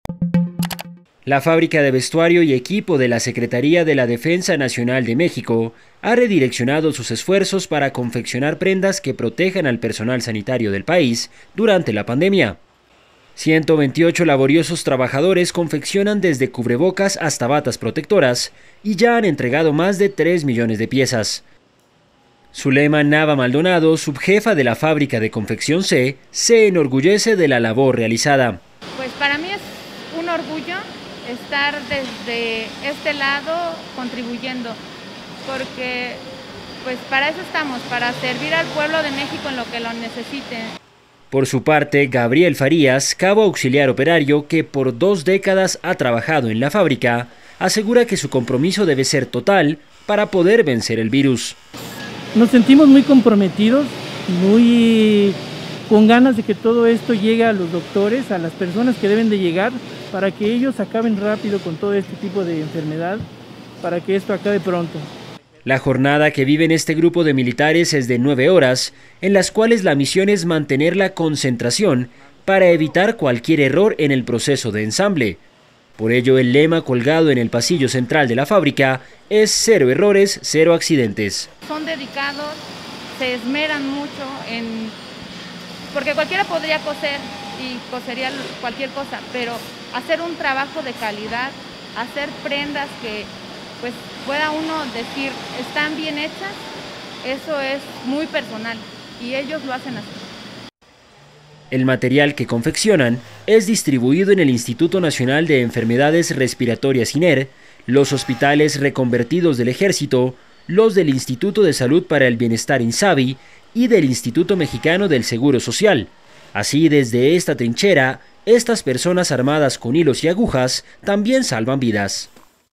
Un batallón detrás de máquinas de coser ayuda a combatir el covid en México
Hace 20 años Gabriel Farías se enroló en este equipo de cosedores del Ejército mexicano que hoy confecciona desde cubrebocas hasta batas protectoras destinadas a proteger al personal de salud durante la pandemia.

¡Visítenos!